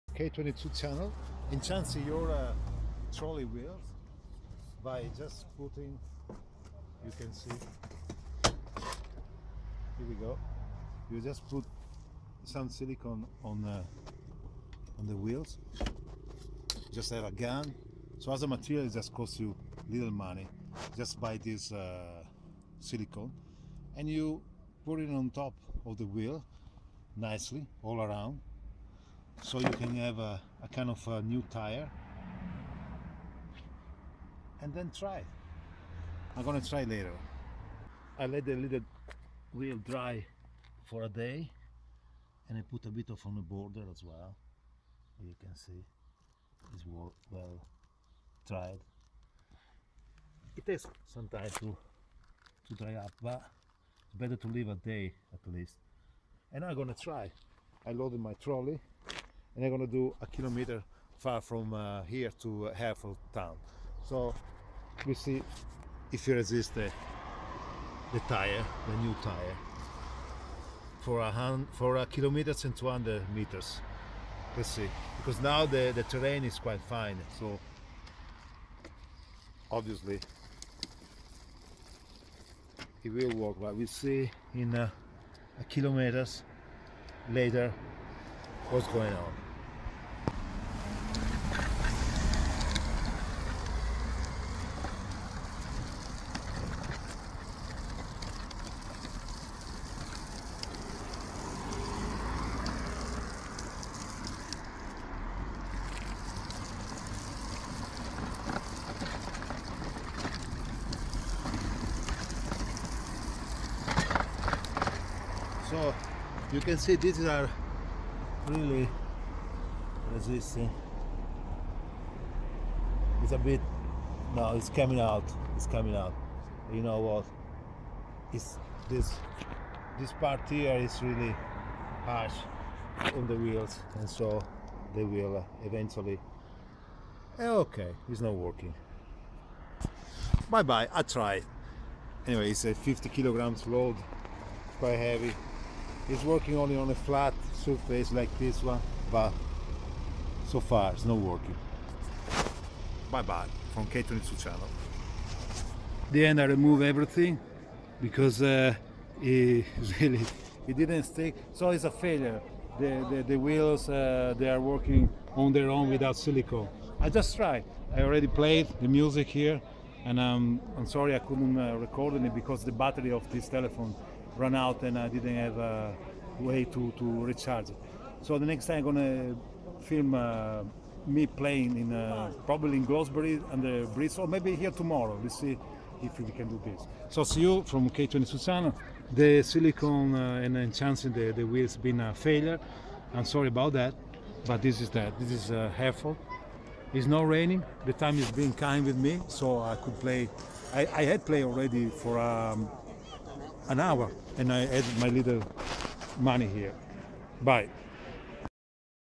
silicon tyres on my trolley weels (failure!)
Trolley carries up to 80kg but those Little weels are only good on flat surfaces therefore I tryed to enchance the weels by adding some silicon on top...it was a failure😊 after 200meters of rough terrain the silicon went away! Any idea? You are welcome 👍